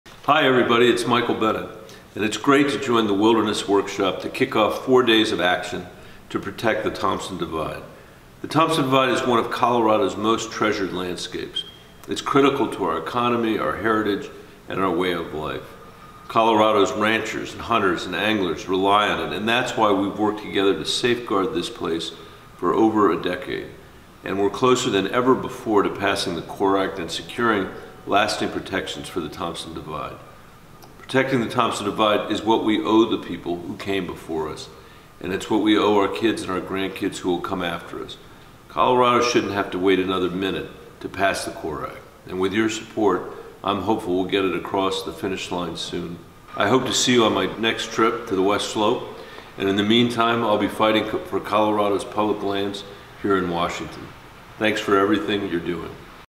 Senator Bennet kicks off "Spring into Action for the Thompson Divide"
What better way to usher in a new season than to celebrate a beloved landscape in our backyard – the Thompson Divide! Protecting the Divide has been a community priority, and a major focus of Wilderness Workshop's work, for many years - thank you to so many of you whose passion for this place never seems to run out.

As spring begins anew, we invite you to join us for “Spring into Action for the Thompson Divide,” four days of actions our community can take to protect this treasured landscape.